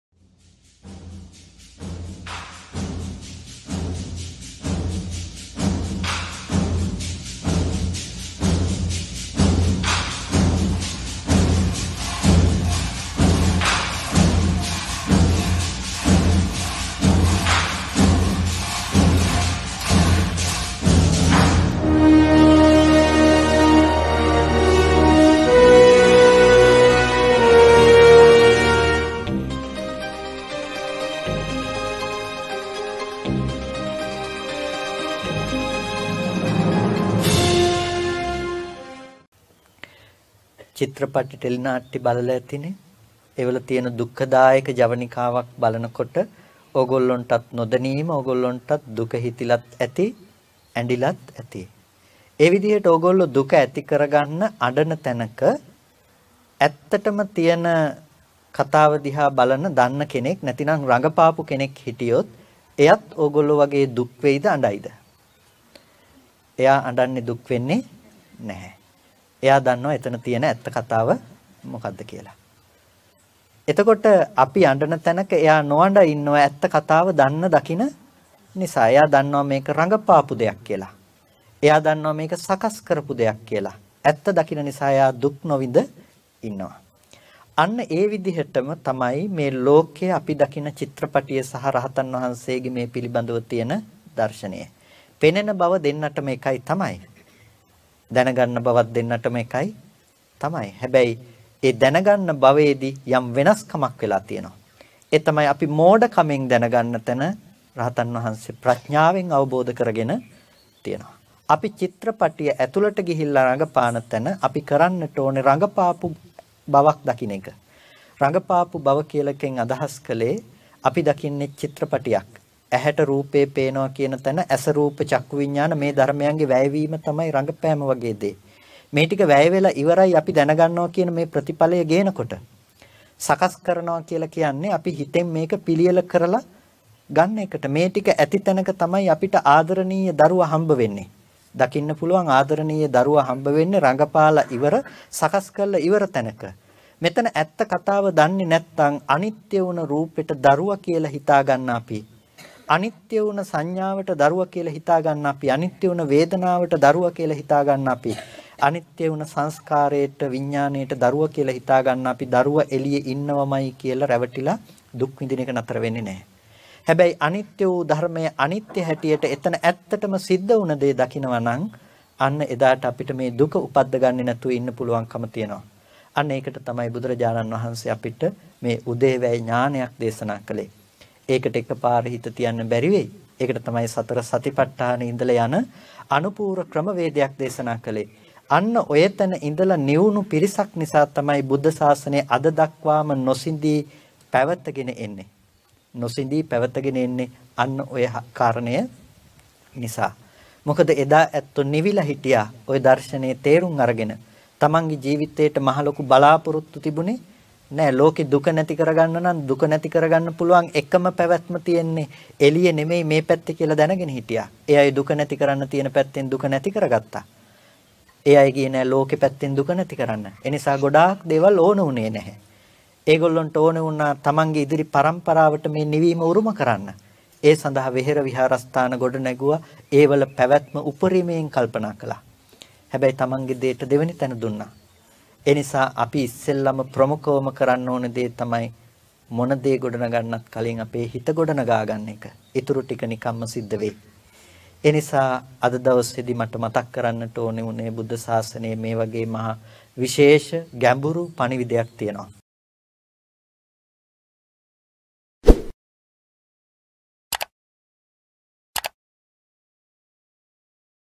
ONE OF THE DEEP MESSAGES OF THE DISPENSION OF BUDDHA
To bring true happiness, one must control ones mind, not physicality if you can control and discipline your mind you will find the way to understand truth and all wisdom and virtues will come to you naturally.